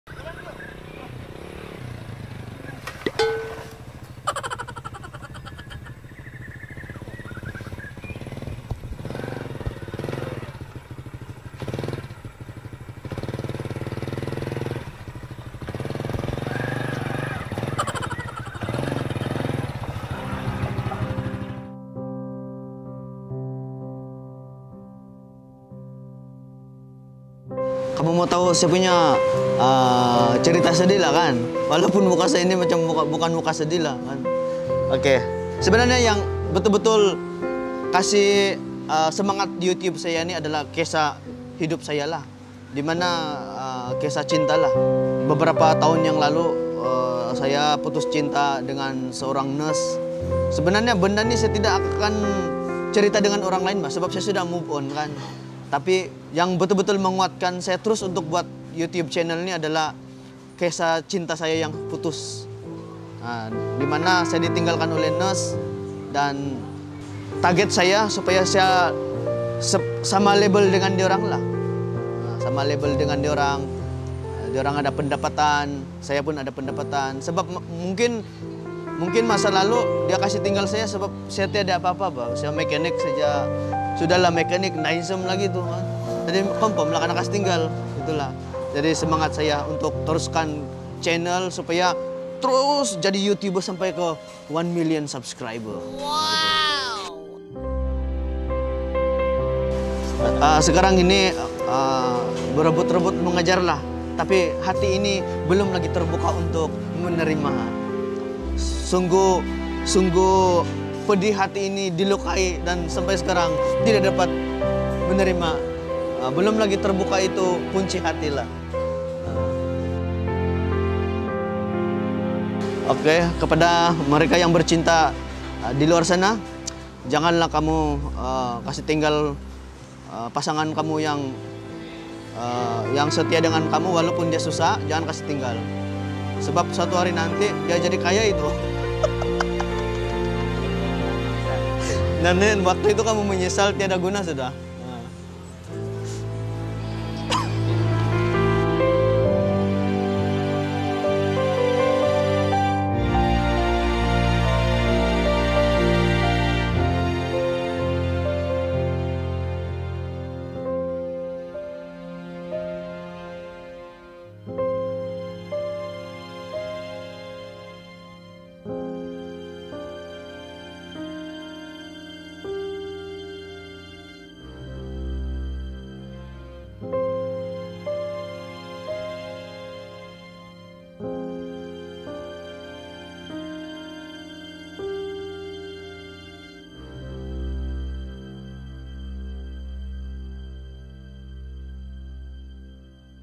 Tumatik Putus Cinta
Tumatik Saulus berkongsi kisah percintaan beliau yang tidak berpanjangan.